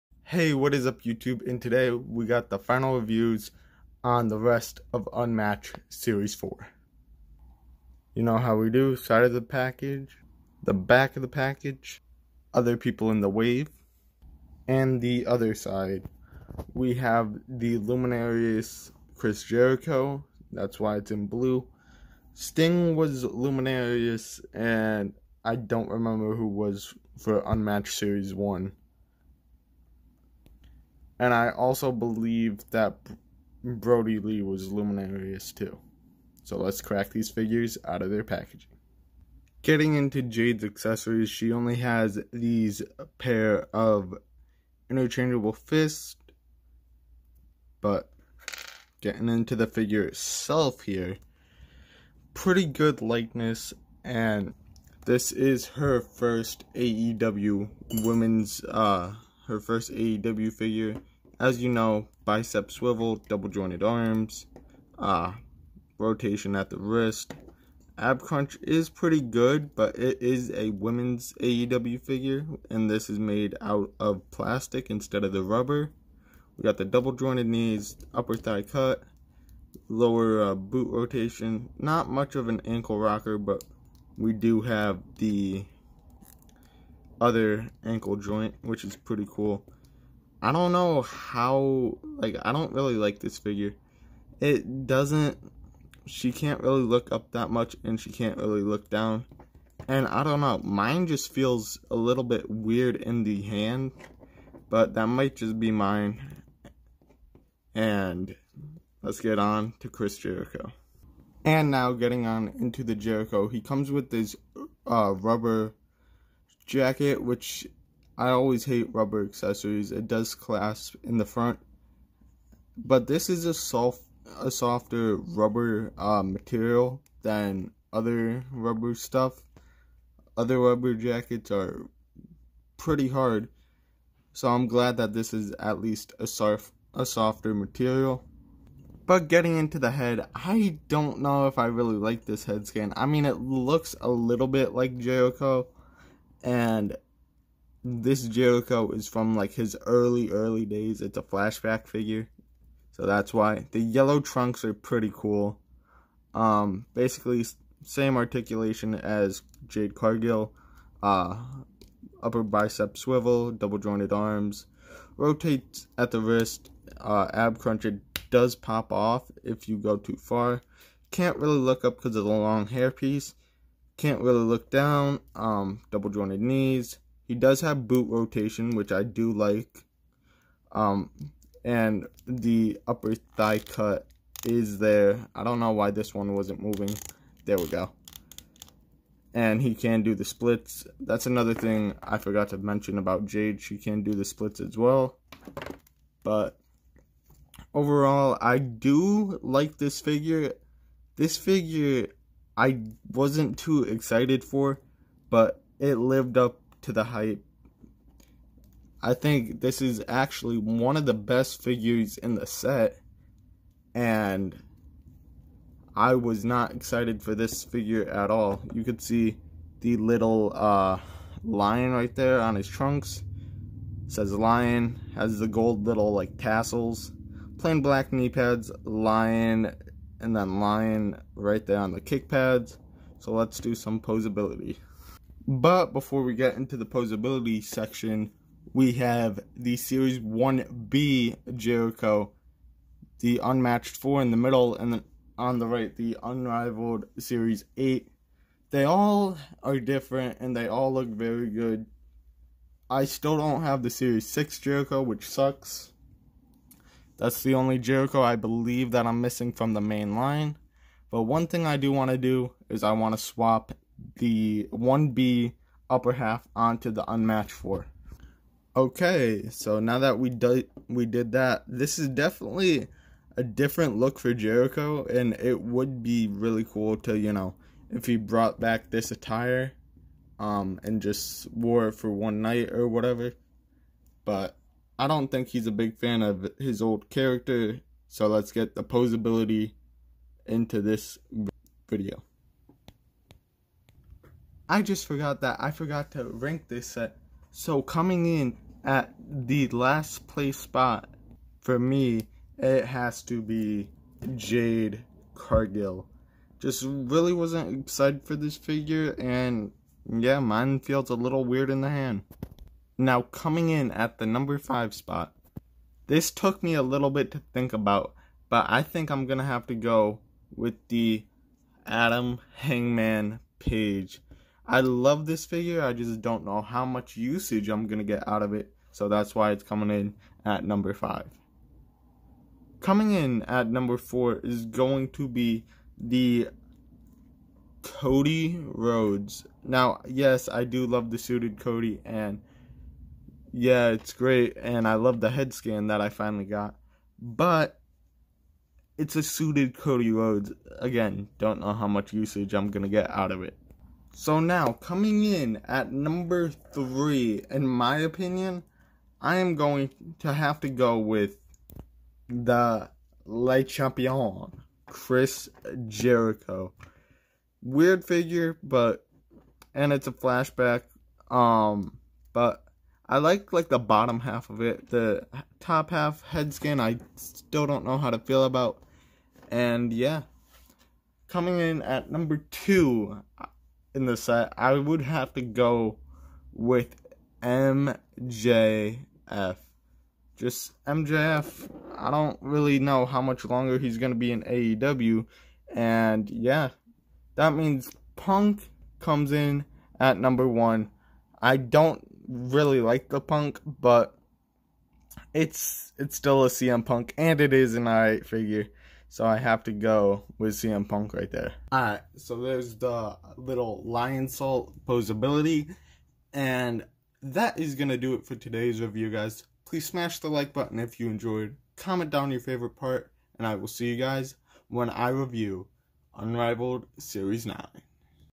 AEW UNMATCHED SERIES 4 CHRIS JERICHO & JADE CARGIL FIGURE REVIEW AND RANKING!
AEW Unmatched Collection series 4 Jade Cargill action figure review!
this new AEW Jazwares figure from the Unmatched Collection